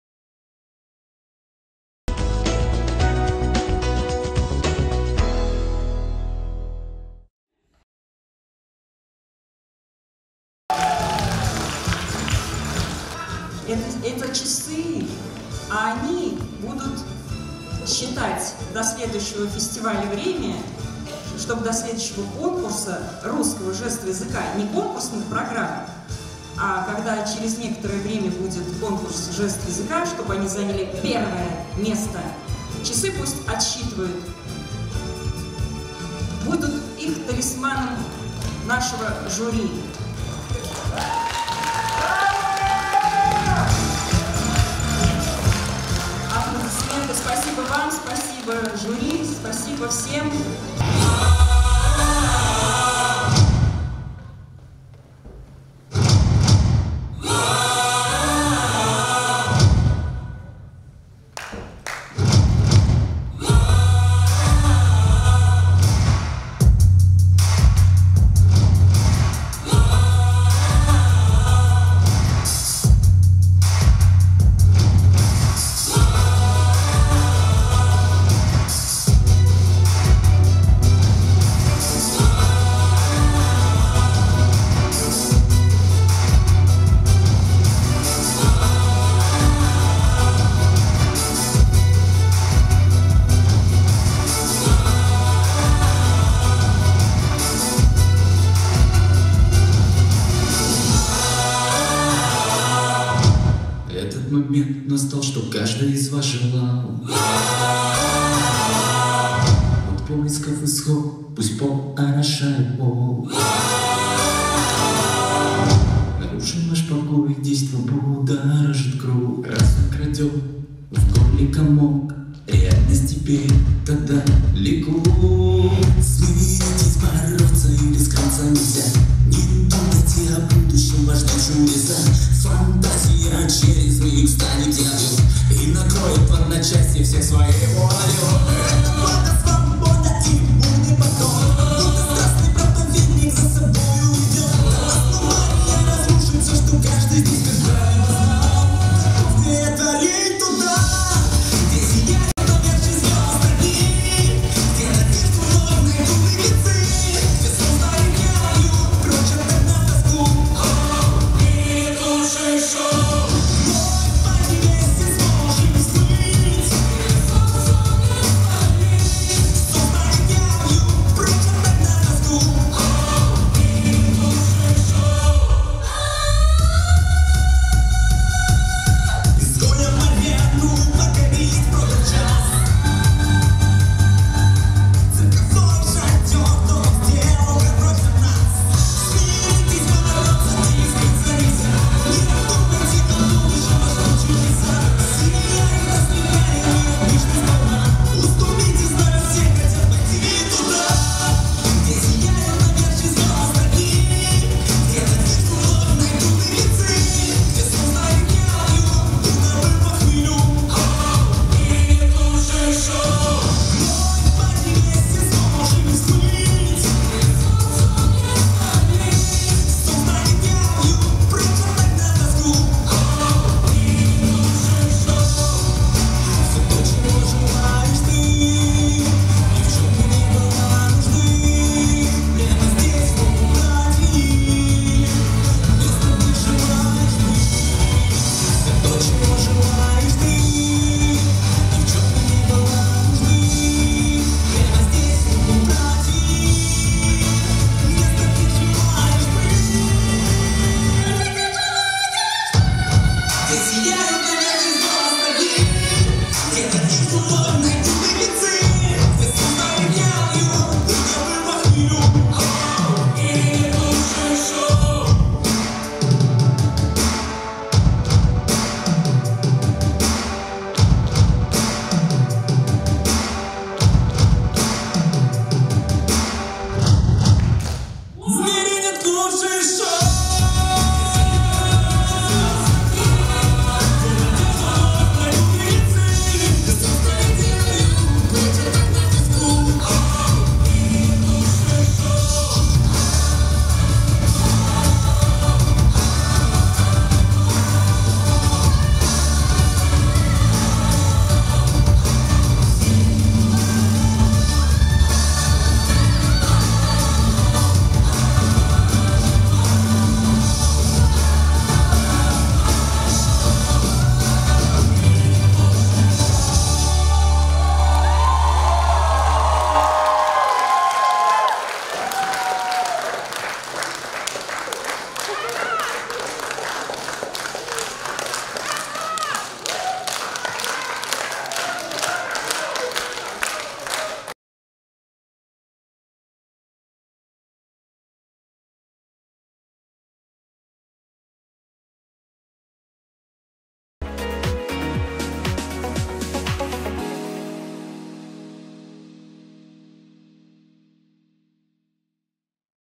"Лучшее шоу" (Челябинское РО ВОГ). СПЕЦПРИЗ
В Казани (16-19 августа 2021) проходил Третий Всероссийский фестиваль жестового языка, посвященный 95-летию ВОГ.

СПЕЦПРИЗ
Ульяна Бородина • Александр Гогулан • Ильшат Гаримуллин • Анастасия Мельник • Вадим Рахимов • Александр Топорищев • Дмитрий Хлыбов • Кристина Якубова
"Лучшее шоу" — Челябинское РО ВОГ